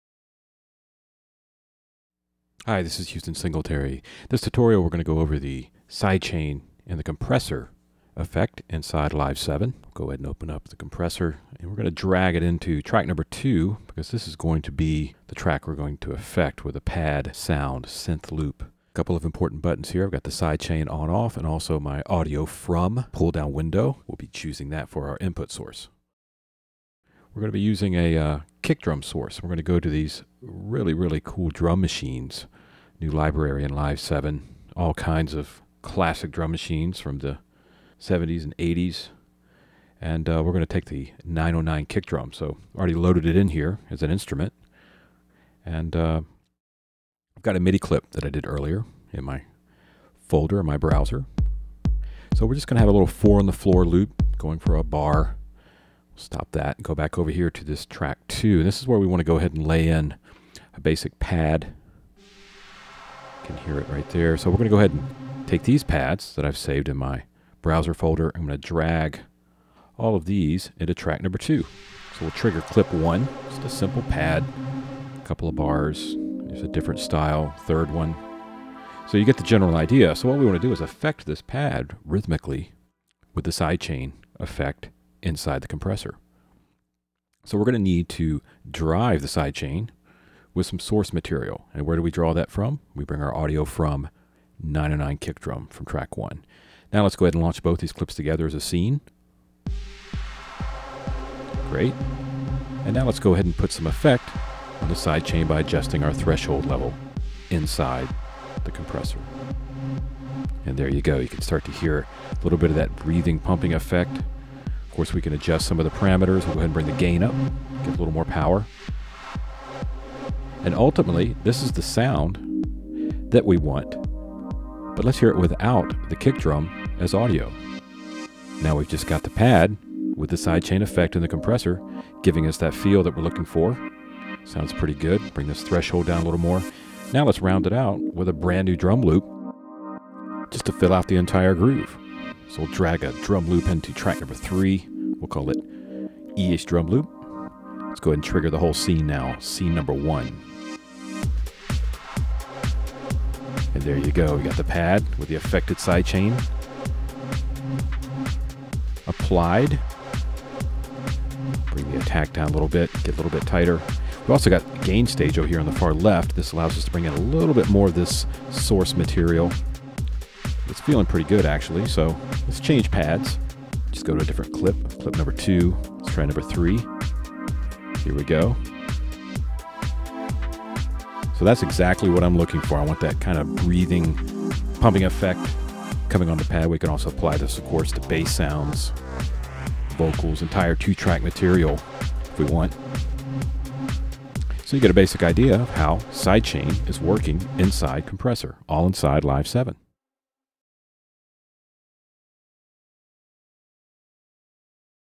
Compressor Sidechain Feature in Ableton Live 7
Huston Singletary takes a look at the Sidechain feature inside the new Compressor in Ableton Live 7.